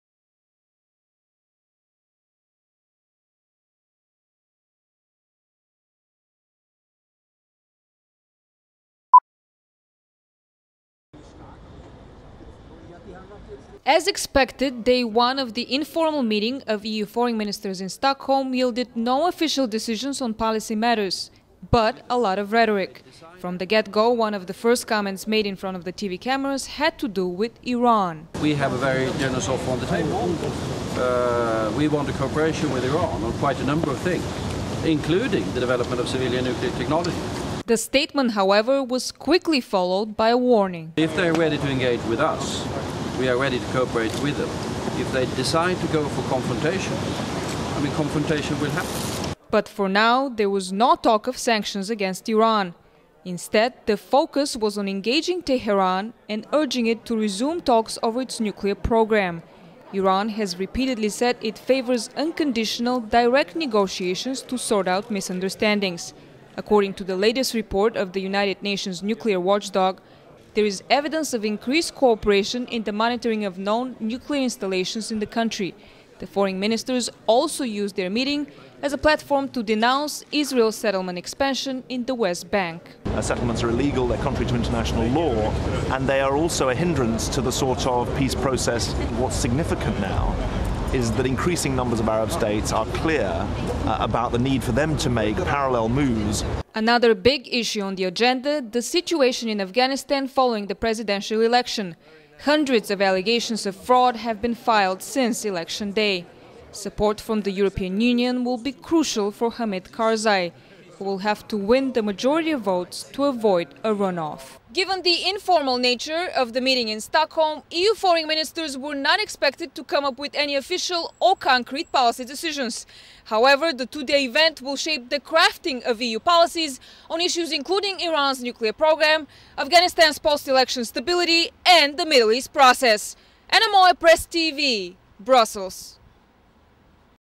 Stockholm Meeting of EU Foreign Ministers
During a two-day informal meeting of EU foreign ministers, Afghanistan and Iran come to the forefront of discussions.

Swedish Foreign Minister Carl Bildt says EU seeks engagement with Iran on a number of issues including nuclear technology.